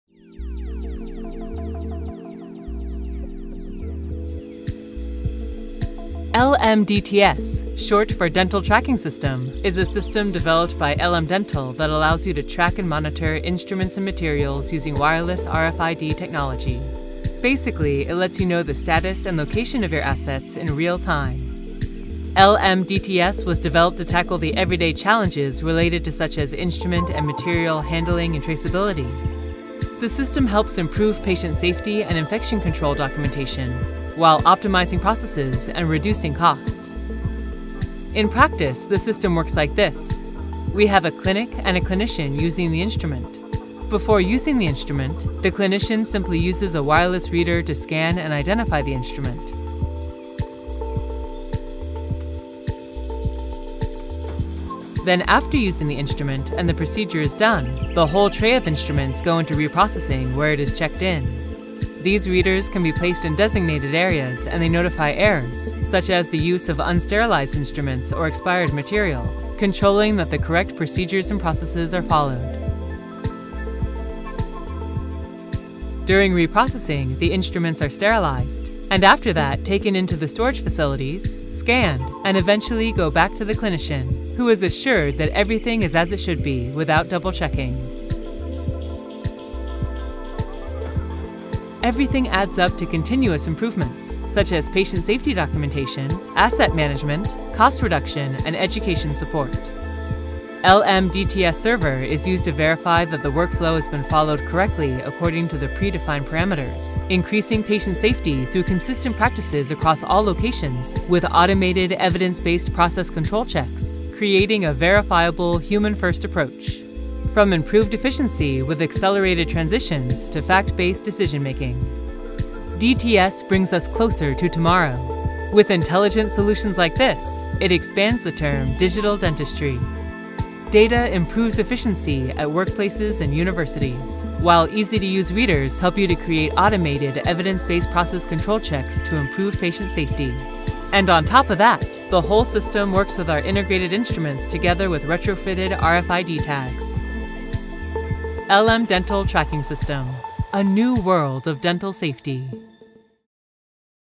LM Dental Tracking System™ - What is LM Dental Tracking System™?
LM DTS™ not only provides valuable data – it allows you to turn vital information into action that makes a difference.

LM Dental Tracking System™ is designed to provide a unified solution in the field of dentistry: via RFID technology, it provides a holistic approach for improved safety, cost-efficiency, and asset management.

Through LM DTS™, dental equipment such as instruments, sensors, handpieces, filling materials, implants, and more from different manufacturers are wirelessly read in a few seconds.

Automating traceability, logistics, and infection control documentation allow staff to work more efficiently and focus both on their tasks and on their patients. IoT in dentistry enables big data gathering and analysis for constant improvement of processes, workflows, AI, and cost-efficiency.

CE marked |
Manufacturer:  LM-Dental™│ LM-Instruments Oy │Norrbyn rantatie 8, FI-21600 Parainen │ Finland